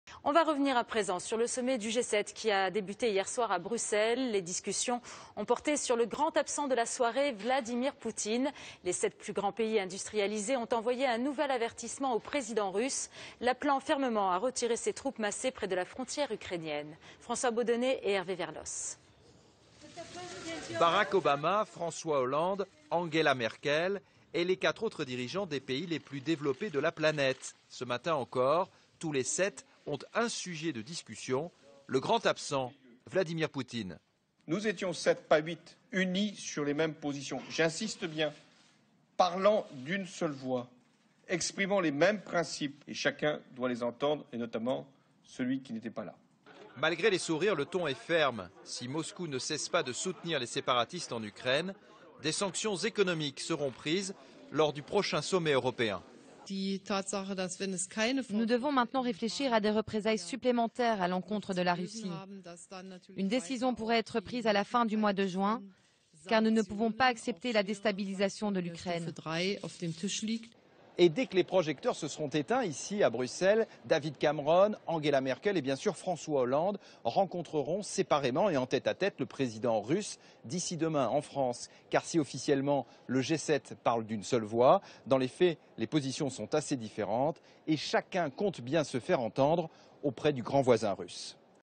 G7 : les dirigeants lancent un avertissement à Vladimir Poutine
Le grand absent du G7 était Vladimir Poutine, qui n'avait pas été convié. Les 7 dirigeants des pays les plus industrialisés de la planète ont appelé la Russie à retirer ses troupes massées le long de la frontière ukrainienne.